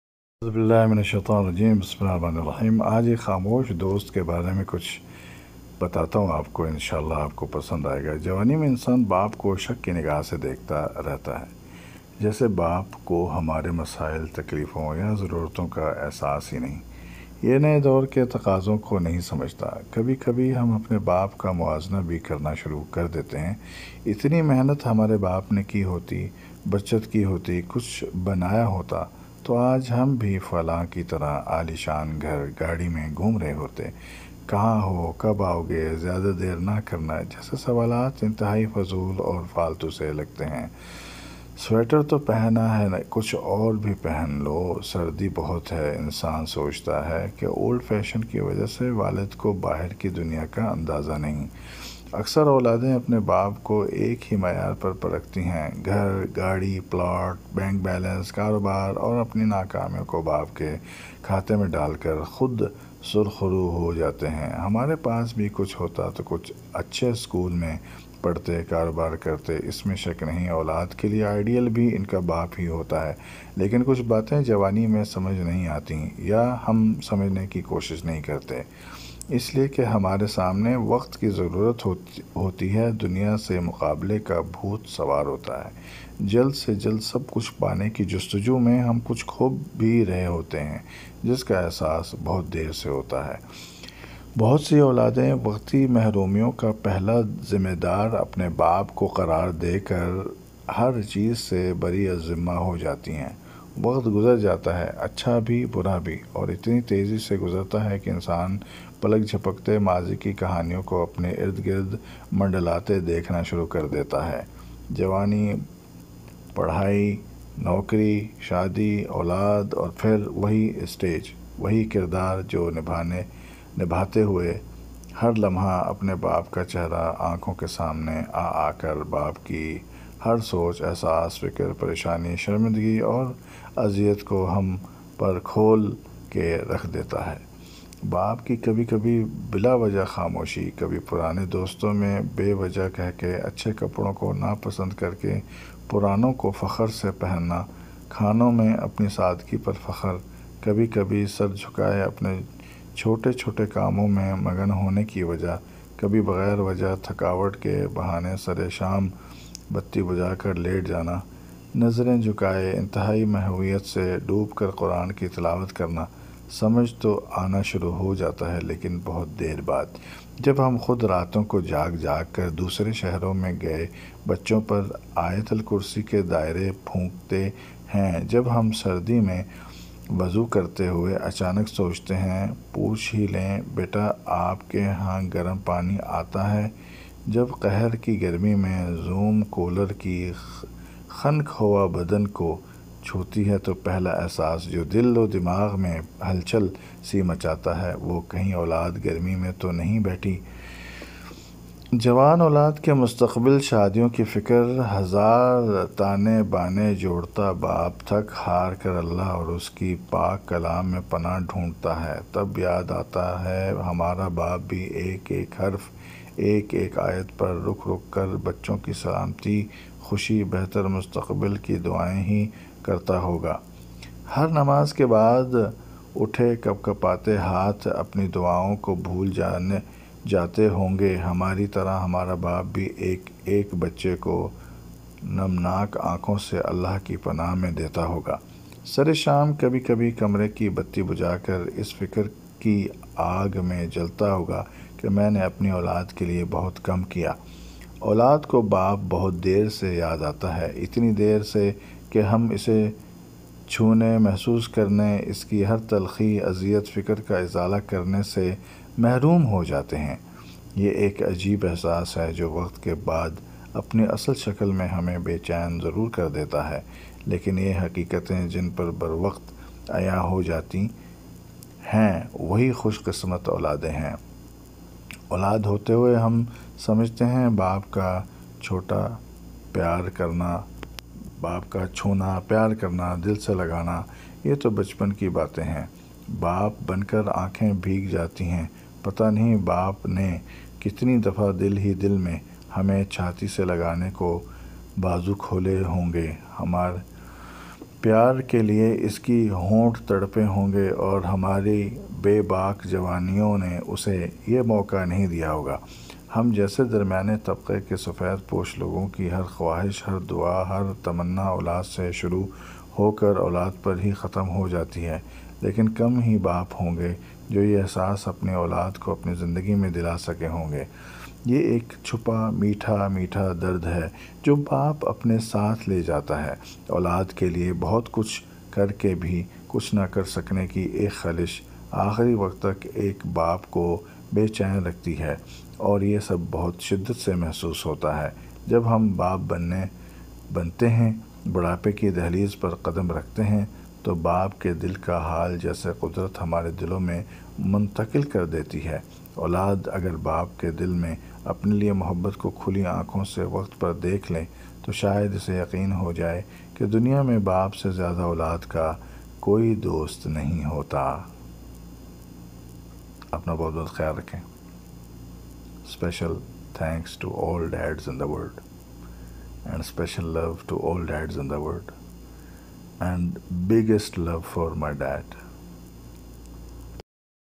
The Father| The Guardian| The Caretaker| The Hero of his Children. Baap. باپ| الأب
Created by Mohammad Amjad
BPR Consultant Trainer.
خاموش دوست

  *جوانی میں انسان باپ کو شک کی نگاہ سے دیکھتا رہتا ہے , جیسے باپ کو ہمارے مسائل ,تکلیفوں یا ضرورتوں کا احساس ہی نہیں . یہ نئے دور کے تقاضوں کو نہیں سمجھتا*

    *کبھی  کبھی ہم اپنے باپ کا موازنہ بھی کرنا شروع کر دیتے ہیں , " اتنی محنت ہمارے باپ نے کی ہوتی , بچت کی ہوتی , کچھ بنایا ہوتا*
      *تو آج ہم بھی ... فلاں کی طرح عالیشان گھر , گاڑی میں گھوم رہے ہوتے "*
    *" کہاں ہو ? کب آؤ گے ? زیادہ دیر نہ کرنا " جیسے سوالات انتہائی فضول اور فالتو سے لگتے ہیں*

    *" سویٹر تو پہنا ہے کچھ اور بھی پہن لو سردی بہت ہے "  انسان سوچتا ہے کہ اولڈ فیشن کی وجہ سے والد کو باہر کی دنیا کا اندازہ نہیں*

    *اکثر اولادیں اپنے باپ کو ایک ہی معیار پر پرکھتی ہیں  , گھر ,گاڑی ,پلاٹ , بینک بیلنس , کاروبار اور اپنی ناکامیوں کو باپ کے کھاتے میں ڈال کر*
    *خود سُرخرو ہو جاتے ہیں "  ہمارے پاس بھی کچھ ہوتا تو اچھے اسکول میں پڑھتے , کاروبار کرتے "*
    *اس میں شک نہیں*
    *اولاد کے لئے آئیڈیل بھی ان کا باپ ہی ہوتا ہے لیکن کچھ باتیں , جوانی میں سمجھ نہیں آتیں یا ہم سمجھنے کی کوشش نہیں کرتے , اس ليے کہ ہمارے سامنے , وقت کی ضرورت ہوتی ہے , دنیا سے مقابلے کا بُھوت سوار ہوتا ہے*
    *جلد سے جلد سب کچھ پانے کی جستجو میں ہم کچھ کھو بھی رہے ہوتے ہیں , جس کا احساس بہت دیر سے ہوتا ہے*

    *بہت سی اولادیں , وقتی محرومیوں کا  پہلا  ذمّہ دار اپنے باپ کو قرار دے کر ,ہر چیز سے بری الزمہ ہو جاتی ہیں*

    *وقت گزر جاتا ہے , اچھا بھی برا بھی , اور اتنی تیزی سے گزرتا ہے کہ انسان پلک  جھپکتے ماضی کی کہانیوں کو اپنے ارد گِرد منڈلاتے دیکھنا شروع کر دیتا ہے*

     *جوانی , پڑھائی , نوکری , شادی , اولاد اور پھر وہی اسٹیج ، وہی کردار , جو نِبھاتے ہوئے , ہر لمحہ , اپنے باپ کا  چہرہ آنکھوں کے سامنے آ آ کر , باپ کی  ہر سوچ , احساس , فکر , پریشانی , شرمندگی اور اذیّت کو ہم پر کھول کے رکھ دیتا ہے*

    *باپ کی کبھی کبھی بلا وجہ خاموشی , کبھی پرانے دوستوں میں بے وجہ قہقہے*
    *اچھے کپڑوں کو ناپسند کرکے ,پرانوں کو فخر سے پہننا , کھانوں میں اپنی سادگی پر فخر*
    *کبھی کبھی سر جُھکائے اپنے چھوٹے چھوٹے کاموں میں مگن ہونے کی وجہ*
    *کبھی بغیر وجہ تھکاوٹ کے بہانے سرِ شام بتّی بجھا کر لیٹ جانا*

    *نظریں جھکائے , انتہائی محویت سے , ڈوب کر قران کی تلاوت کرنا ,  سمجھ تو آنا شروع ہو جاتا ہے لیکن بہت دیر بعد*

    *جب ہم خود راتوں کو جاگ جاگ کر , دوسرے شہروں میں گئے بچوں پر آیت الکرسی کے دائرے پھونکتے ہیں*
    *جب ہم سردی میں وضو کرتے ہوئے اچانک سوچتے ہیں , پًوچھ ہی لیں " بیٹا  آپ کے ہاں  گرم پانی آتا ہے ؟ "*

    *جب قہر کی گرمی میں رُوم کولر کی خنک ہوا بدن کو چُھوتی ہے تو پہلا احساس جو دل و دماغ میں ہلچل سی مچاتا ہے , وہ " کہیں اولاد گرمی میں تو نہیں بیٹھی "*

    *جوان اولاد کے مستقبل , شادیوں کی فکر , ہزار تانے بانے جوڑتا باپ , تھک ہار کر اللّه اور اس کی پاک کلام میں پناہ ڈھونڈتا ہے*
    *تب یاد آتا ہے , ہمارا باپ بھی ایک ایک حرف , ایک ایک آیت پر , رُک رْک کر , بچوں کی سلامتی , خوشی , بہتر مستقبل کی دعائیں ہی کرتا ہوگا*

    *ہر نماز کے بعد , اُٹھے کپکپاتے ہاتھ ,اپنی دعاؤں کو بھول جاتے ہونگے , ہماری طرح ہمارا باپ بھی ایک ایک بچے کو , نمناک آنکھوں سے اللّه کی پناہ میں دیتا ہوگا*

    *سرِ شام کبھی کبھی , کمرے کی بتّی بُجھا کر , اس فکر کی آگ میں جلتا ہوگا کہ میں نے اپنی اولاد کے لئے بہت کم کِیا ؟*

    *اولاد کو باپ بہت دیر سے یاد آتا ہے , اتنی دیر سے کہ ہم اسے چْھونے , محسوس کرنے , اسکی ہر تلخی , اذیت , فکر کا ازالہ کرنے سے محروم ہو جاتے ہیں*

     *یہ ایک عجیب احساس ہے , جو وقت کے بعد اپنی اصل شکل میں ہمیں بےچین ضرور کرتا ہے*
    *لیکن یہ حقیقتیں جن پر بروقت عیاں ہو جائیں وہی خوش قسمت اولادیں ہیں*

    *اولاد ہوتے ہوئے ہم سمجھتے ہیں
 باپ کا چھونا , پیار کرنا , دل سے لگانا , یہ تو بچپن کی باتیں ہیں*

    *باپ بن کر آنکھیں بھیگ جاتی ہیں*
    *پتہ نہیں باپ نے کتنی دفعہ دل ہی دل میں , ہمیں چھاتی سے لگانے کو بازو کھولے ہونگے ؟*
    *پیار کے لئے اس کے ہونٹ تڑپے ہونگے*
    *اور ہماری بے باک جوانیوں نے اسے یہ موقع نہیں دیا ہوگا*

    *ہم جیسے درمیانے طبقے کے سفید پوش لوگوں کی*
    *ہر خوائش , ہر دعا , ہر تمنا , اولاد سے شروع ہو کر , اولاد پر ہی , ختم ہو جاتی ہے*

    *لیکن کم ہی باپ ہونگے جو یہ احساس اپنی اولاد کو اپنی زندگی میں دلا سکے ہوں*
    *یہ ایک چُھپا ,میٹھا میٹھا درد ہے جو باپ اپنے ساتھ لے جاتا ہے*
    *اولاد کے  ليے, بہت کچھ کر کے بھی کچھ نہ کرسکنے کی ایک خلش , آخری وقت تک ایک باپ کو بے چین رکھتی ہے , اور*
    *یہ سب بہت شدت سے محسوس ہوتا ہے*
    *جب ہم باپ بنتے ہیں . بڑھاپے کی دہلیز پر قدم رکھتے ہیں*
    *تو باپ کے دل کا حال جیسے قدرت ہمارے دلوں میں منتقل کر دیتی ہے*

    *اولاد اگر باپ کے دل میں اپنے لئے محبت کو کھلی آنکھوں سے , وقت پر دیکھ لے*
    *تو شاید اسے یقین ہو جائے کہ دنیا میں باپ سے زیادہ اولاد کا کوئی دوست نہیں ہوتا* 🤔🤔